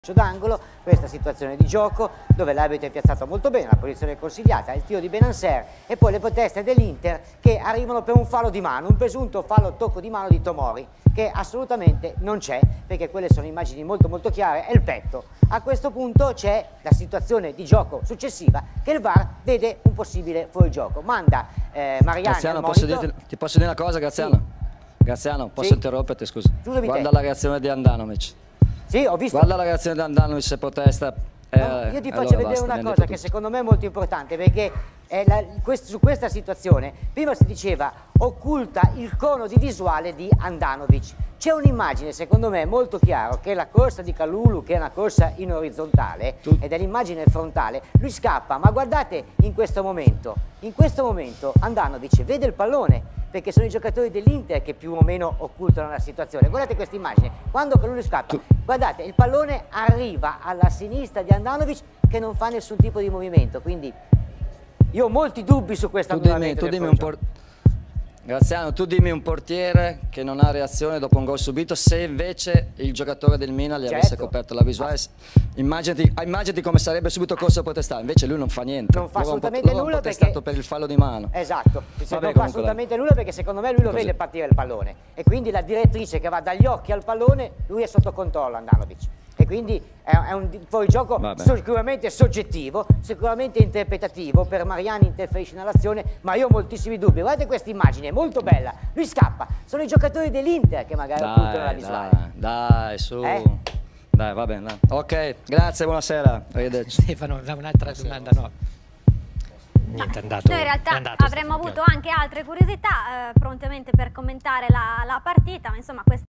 CESARI sul GOL di BENNACER | PIOLI FURIOSO abbandona lo studio
L'Inter vince 3-0 il derby di Coppa Italia e vola in finale, ma il Milan recrimina per un episodio che poteva riaprire l'incontro, il gol di Bennacer annullato al 64' dal VAR per un fuorigioco di Kalulu, che avrebbe ostruito la visuale di Handanovic. La decisione di Mariani non ha convinto i rossoneri ma non solo, anche all'estero non è stata apprezzata. Lucas Hernandez, difensore del Bayern Monaco e fratello gemello del terzino milanista Theo, non ha usato mezzi termini su Instagram: "Scandaloso", con una faccina che ride e i colori rossoneri.